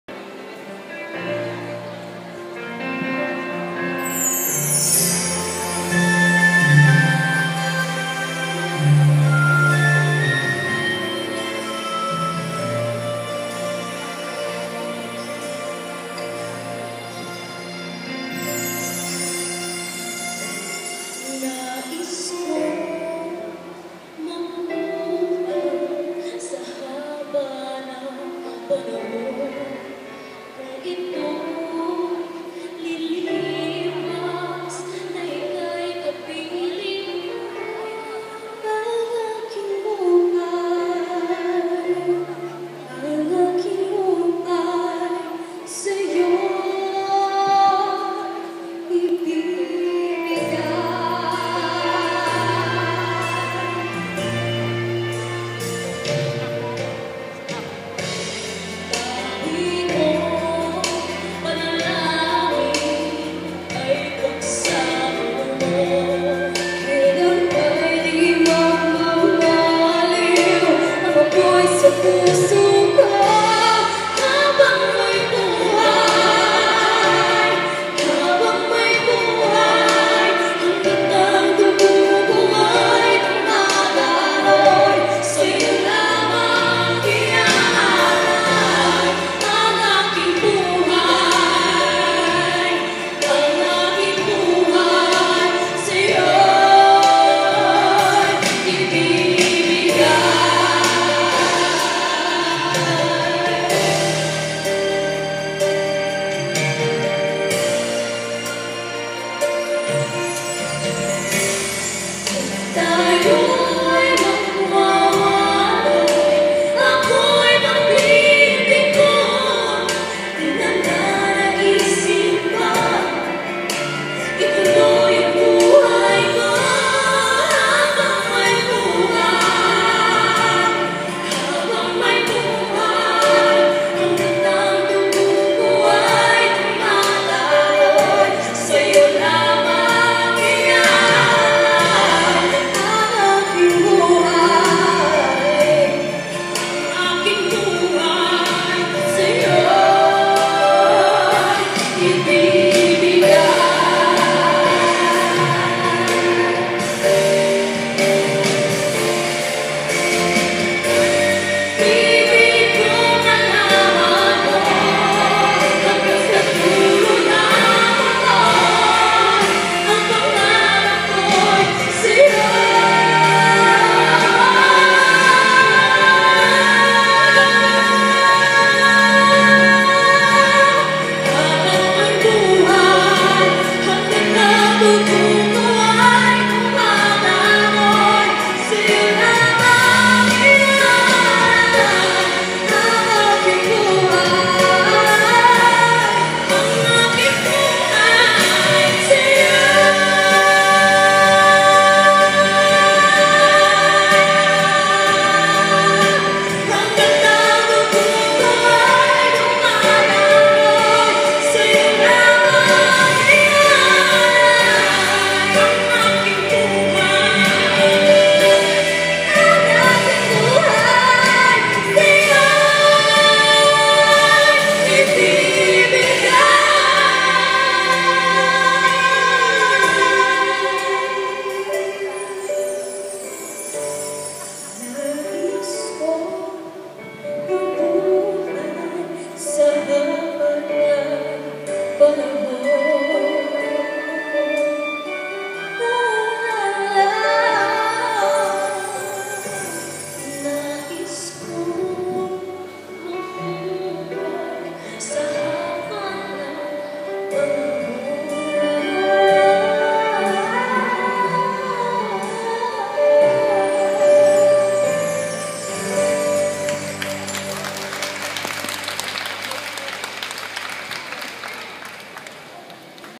2014 PRISAA Regional Champion in Vocal Duet Pop
PAULINIANS: REGIONAL CHAMPIONS IN PRISAA VOCAL DUET POP

Last January 9, 2014, Iloilo PRISAA Tertiary Winners traveled to San Jose, Antique for the Western Visayas Regional PRISAA Meet, which is participated by all the provinces in Region VI, which includes Capiz, Kalibo, Antique, and Negros Occidental. Among the group of PRISAA Winners are two of our own, Nikki Angelli O. Minsalan and Mary Lord J. Salinas, who won the VOCAL DUET POP last November 2013 while competing with 7 other private colleges in Iloilo. After the victory last November, our two Paulinians once again brought home the bacon, winning as CHAMPIONS against four other provincial representatives.